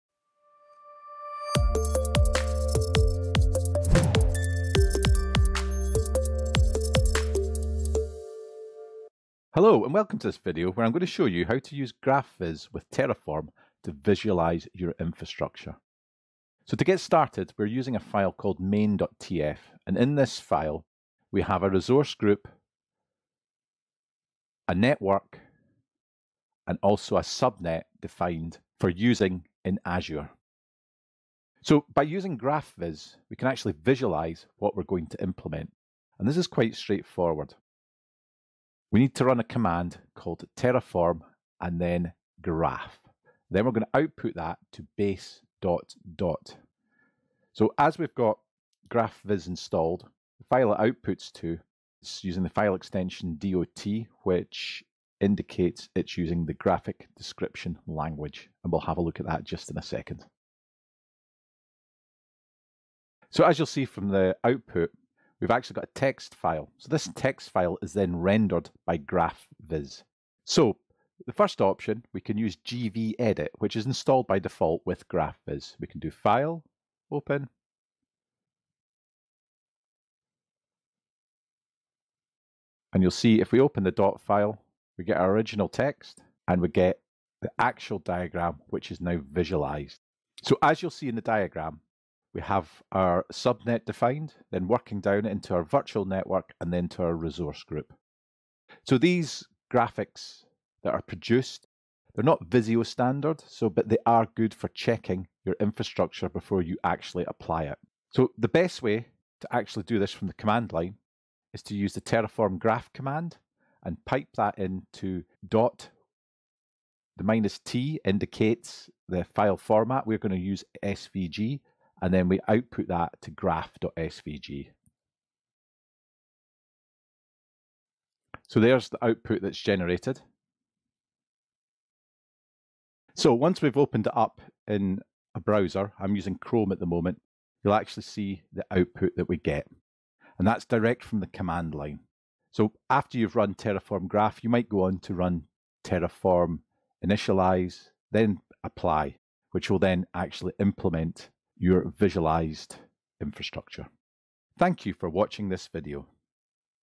How To Use Graphviz With Terraform To Visualize Your Infrastructure
Terraform enables you to create, change and improve infrastructure reliably and predictably, it's open source and lets you create declarative configuration files that can be treated as code. (Infrastructure As Code). In this video, Nick will take you through the steps required to visualise your infrastructure prior to applying it within Azure.
Writing infrastructure as code is great and the recommended path, but how many times have you thought, 'I would like to visualise this, see what my configuration looks like'? Terraform comes with this functionality built in, using Graphviz (you need to install this) and the terraform 'graph' command you will generate a SVG format file to view your infrastructure. A great artefact to store with your code !

Add graphviz to your path C:\Program Files (x86)\Graphviz2.38\bin
Azure Subscription
Terraform installed